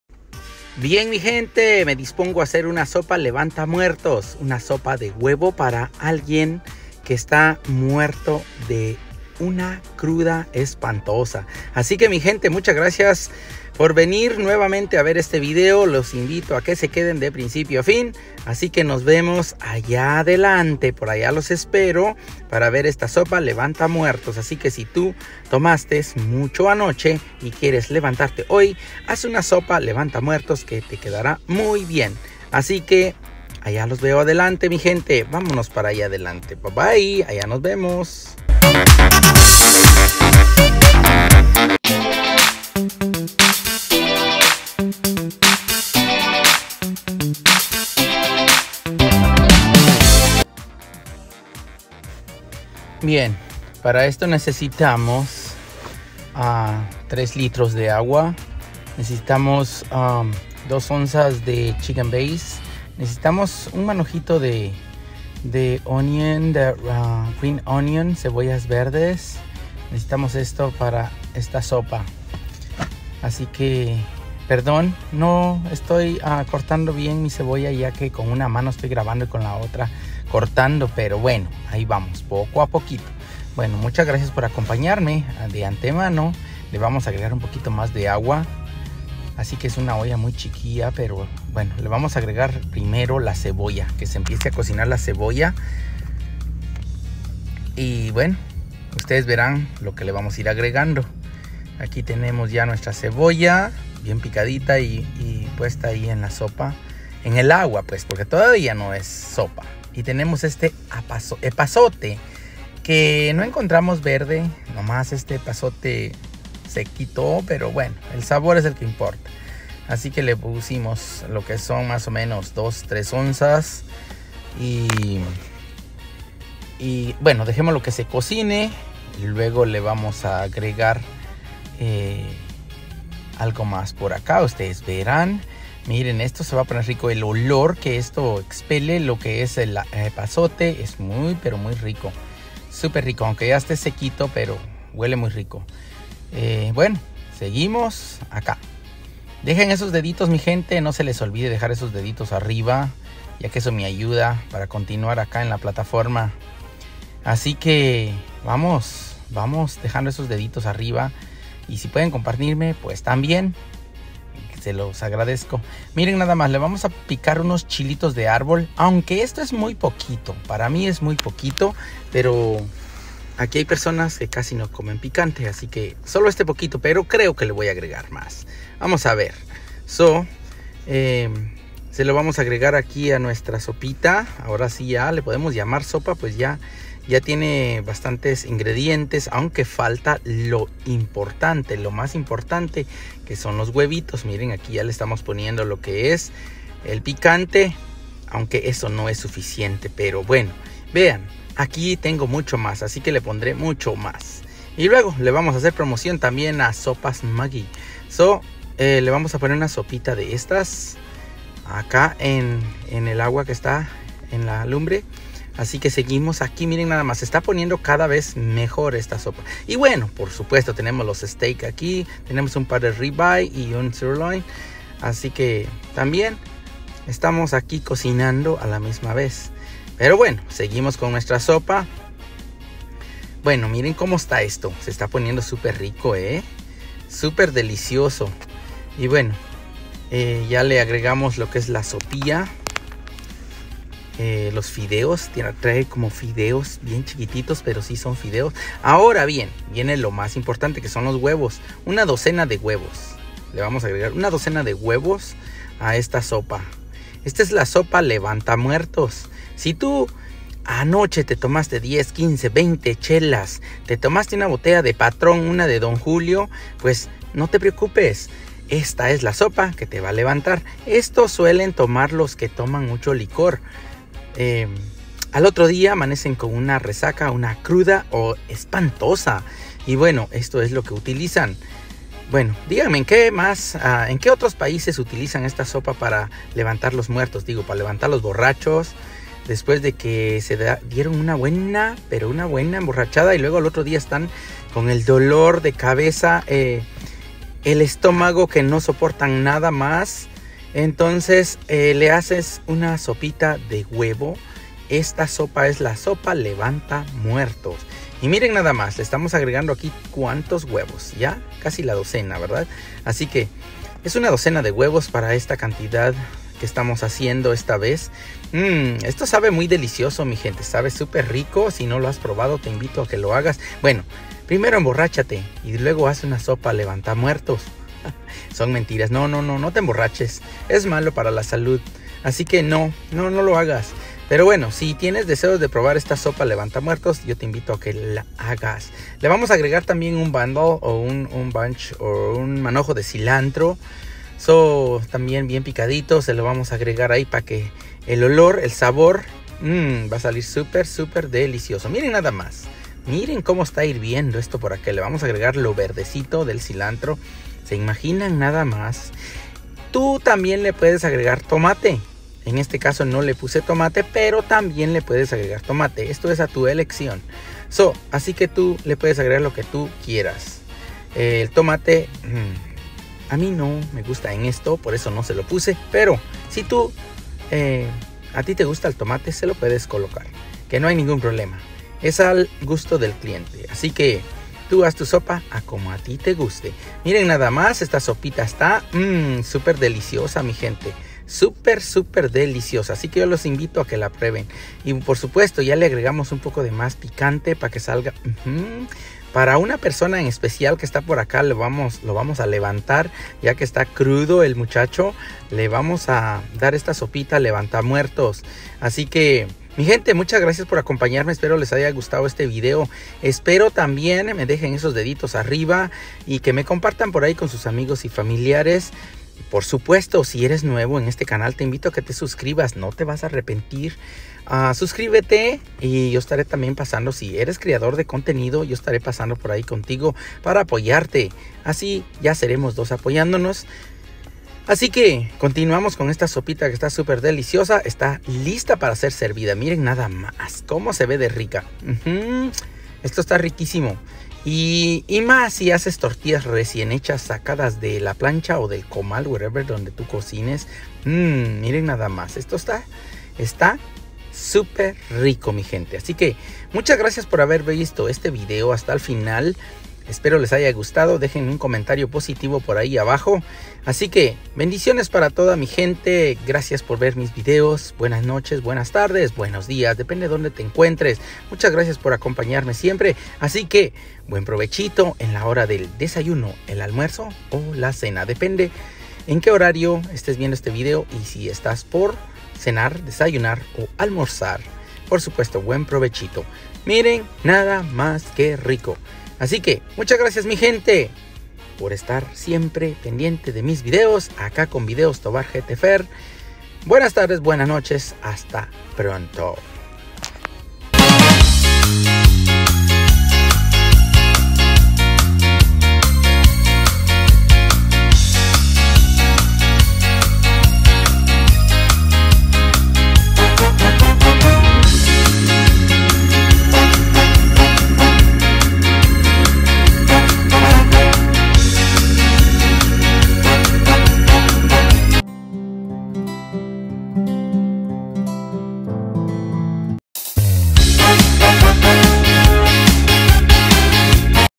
COMO HACER UNA SOPA O CALDO DE HUEVO 🥚 LA CUAL LLAMAMOS SOPA LEVANTA MUERTOS A MI ESTILO 🙃🙃😆
Como preparar la sopa de huevos
Receta:
3 litros de agua / water
8 cebollines or green onion
8 chiles de árbol /hot chili 🌶
6 oz de epazote
1 sopa maggi
12 huevos/eggs
1 oz de sazonador  de pollo/chicken base
Sal al Gusto y una previa borrachera